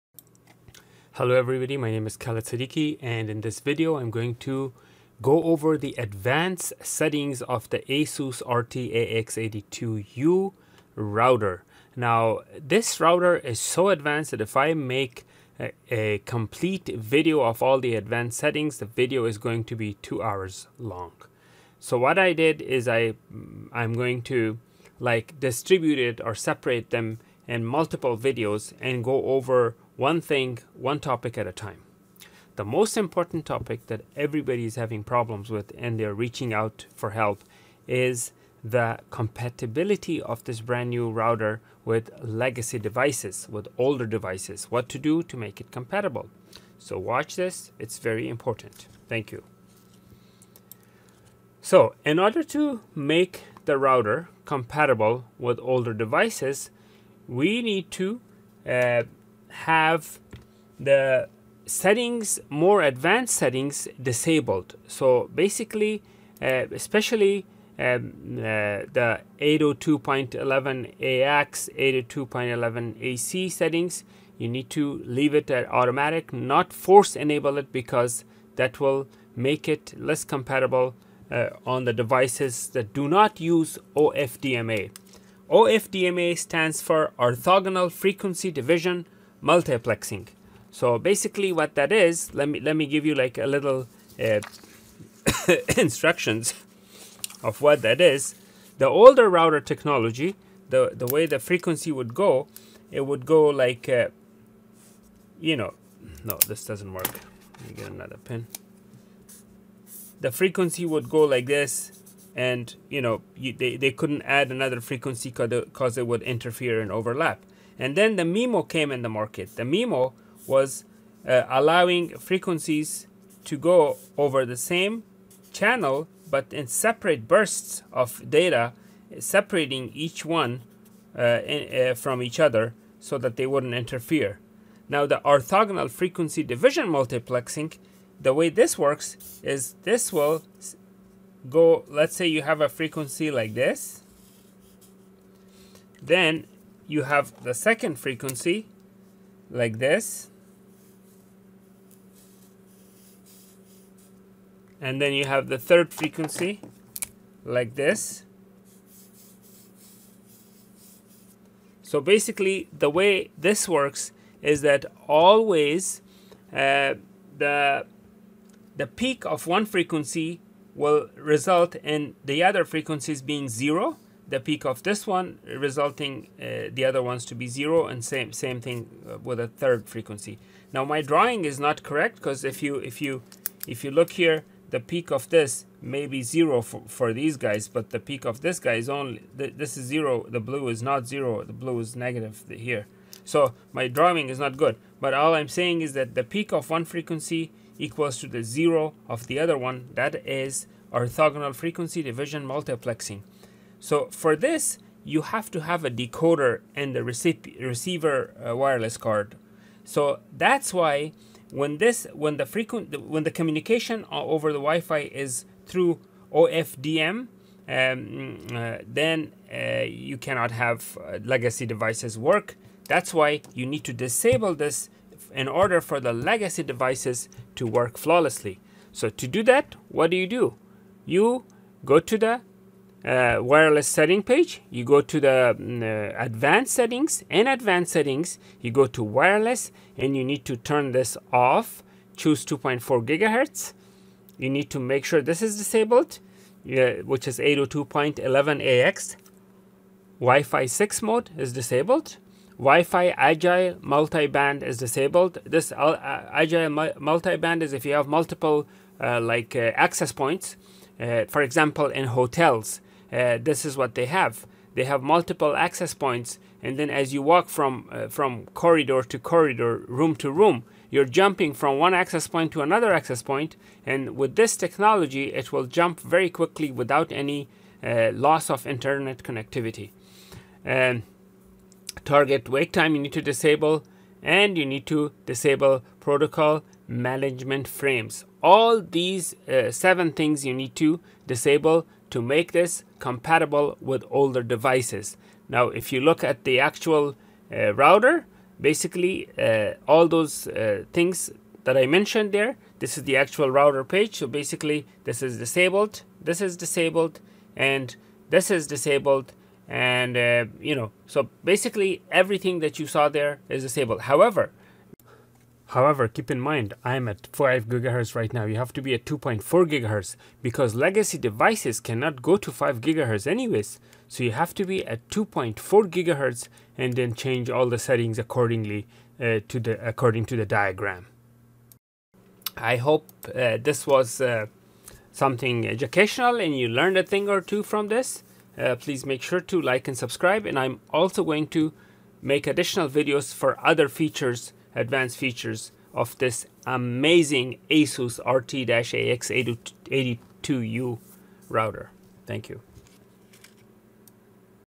Legacy Device Compatibility Advanced Setup On ASUS RT-AX82U AX5400 WiFi Wireless Router
This is Basic Configuration, I will have a more in-depth video for advanced configurations.